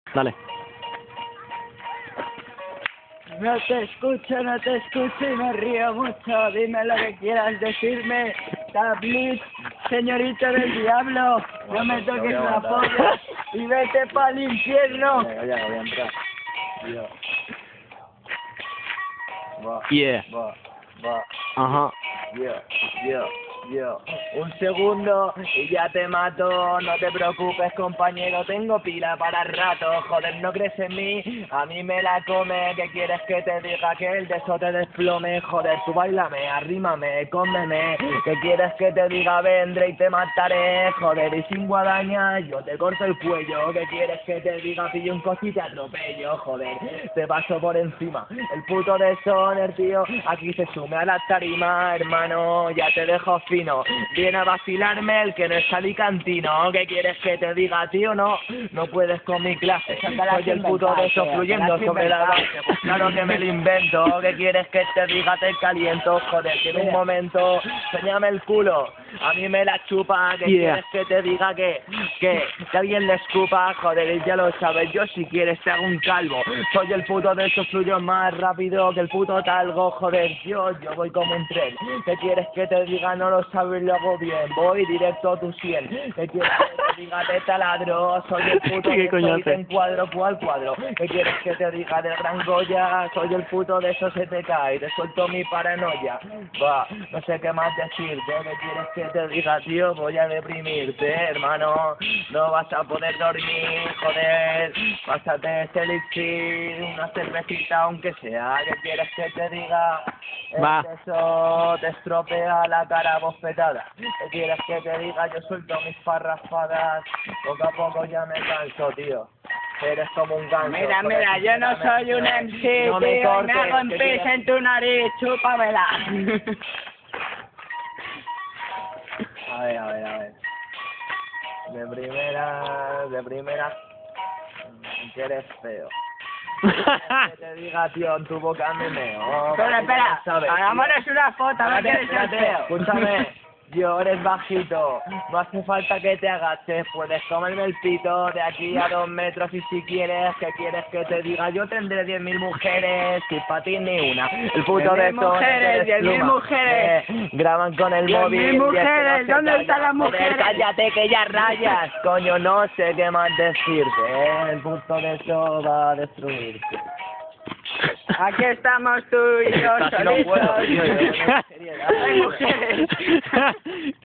Deso feat Tipo dando la nota
Improvisación de rap español en las alemanias.